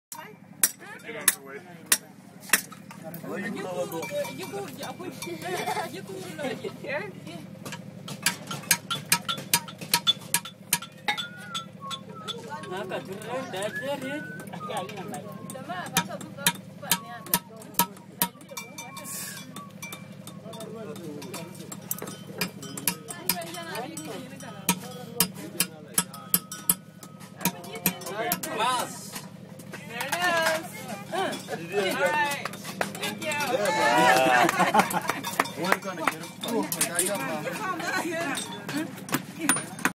Making Bracelets From Bullets in South Sudan: Treana Peake Gets Custom Jewelry (Obakki Foundation)
In South Sudan, Treana came across a vendor making beauty out of war - creating jewelry from bullets. Here, she has one of the Bullets for Peace custom fit to her wrist.